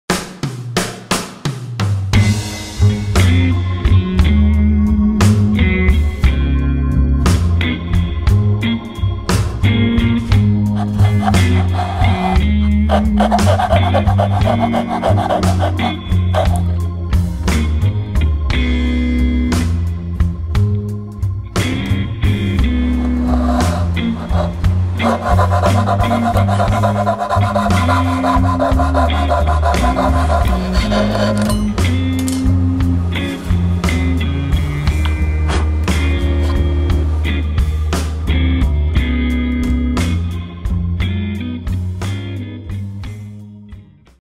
Spiral Blades for Dovetails
Contributing Editor Christopher Schwarz compares the speed and accuracy of coping saw blades vs. spiral fretsaw blades when removing dovetail waste.